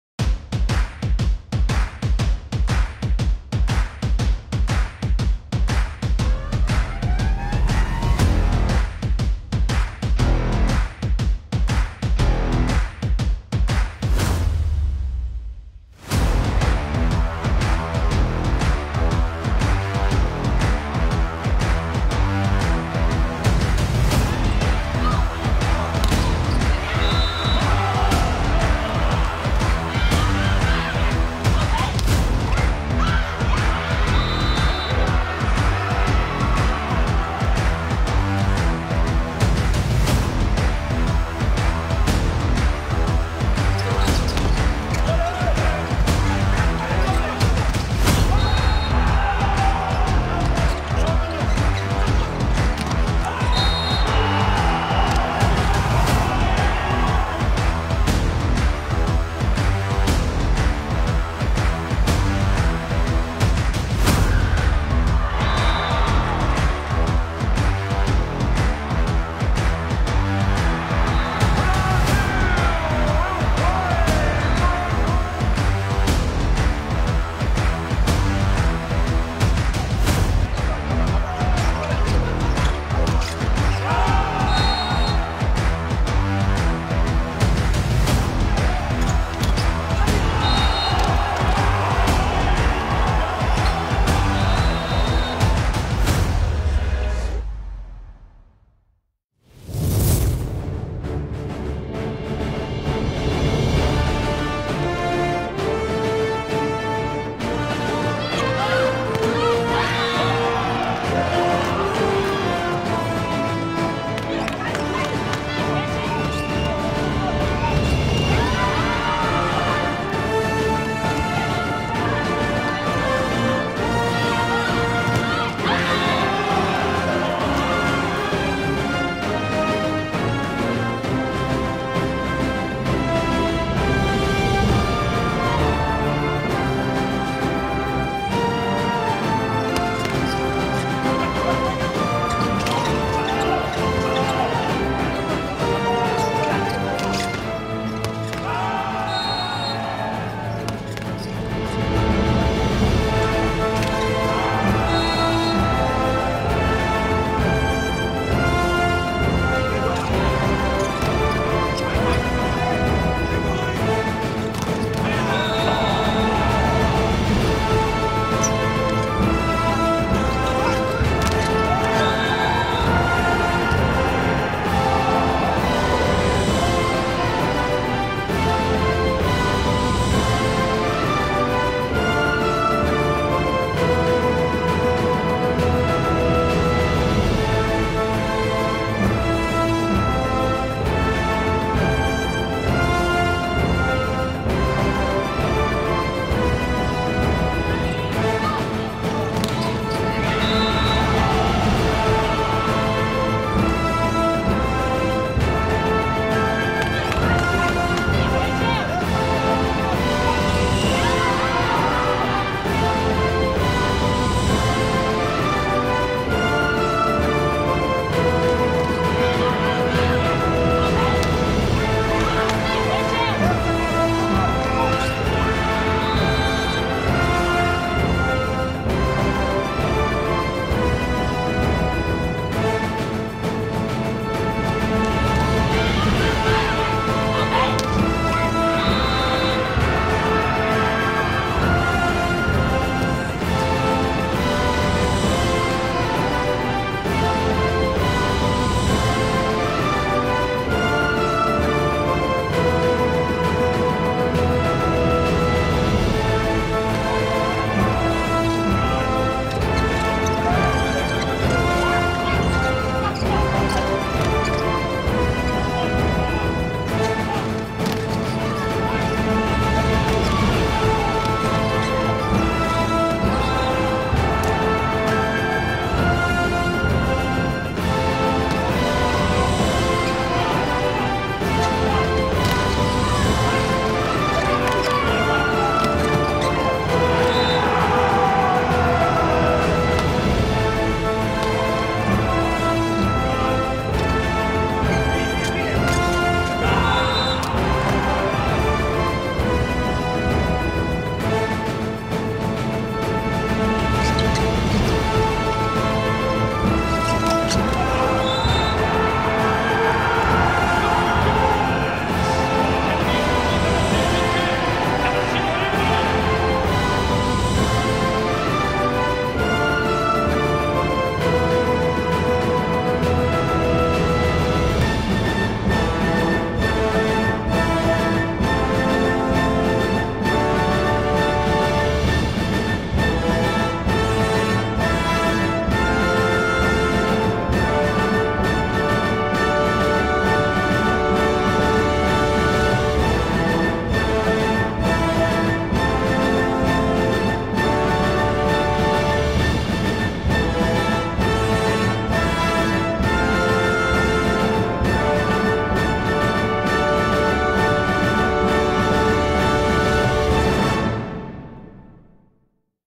🏐 Volleyball finals highlights from Tokyo2020
The Volleyball finals at Tokyo2020 had it all! World number ones USA beat Brazil in the women's final to become Olympic champions, while France defeated ROC to take the men's volleyball gold. Enjoy watching the Volleyball finals highlights from Tokyo2020!